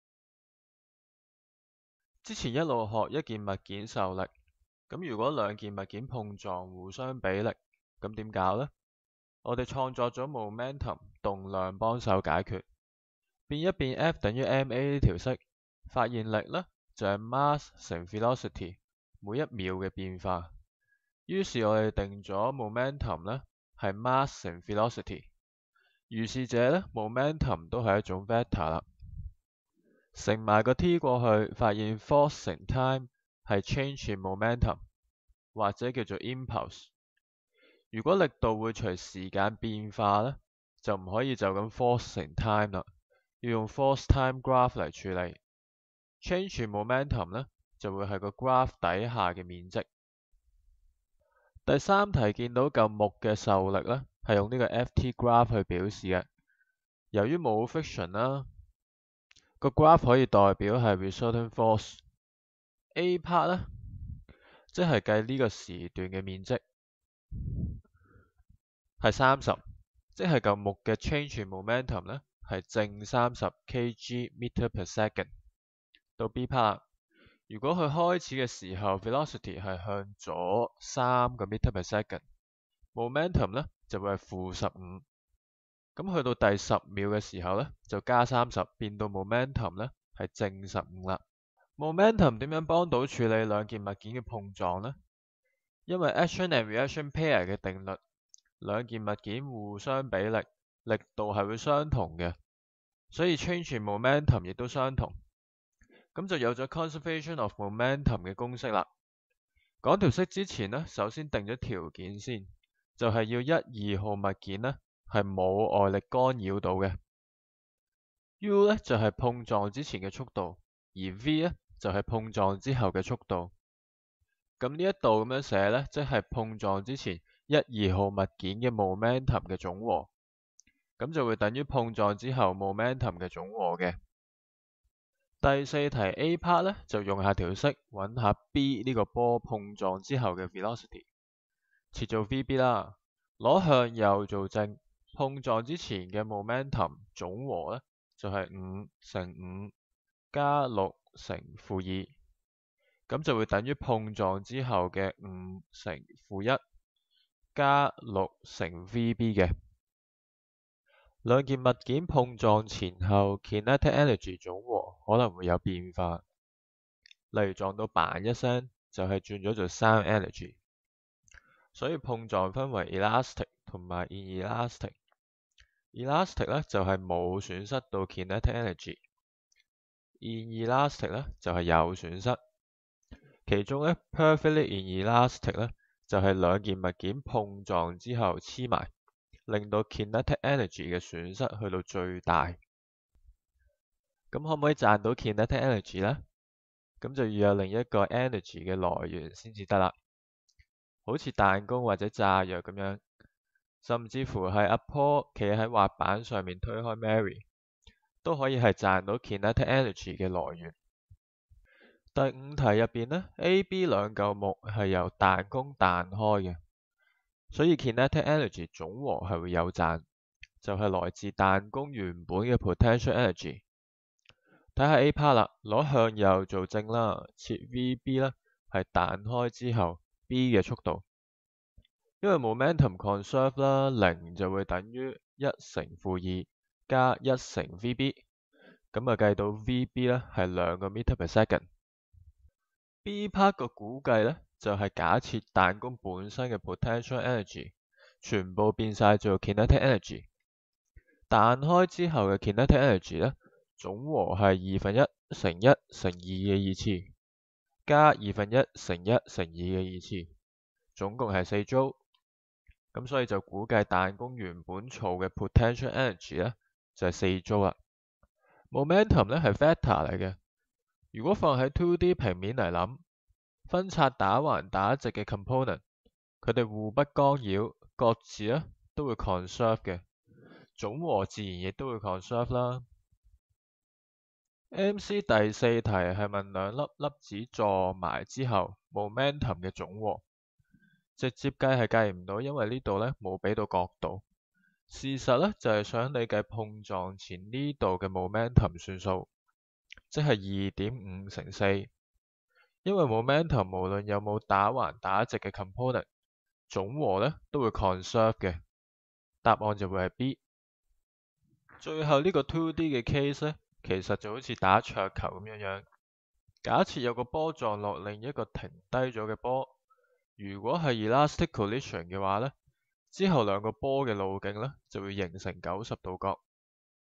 【HKDSE 逐課快講】《字幕》Phy Topic II-5 Momentum 動量 -- 光速DSE
跟足DSE分課 + 完整筆記 + 仲有字幕

學校上堂跟唔到, 自己睇書睇唔明... ? 睇下精華先啦!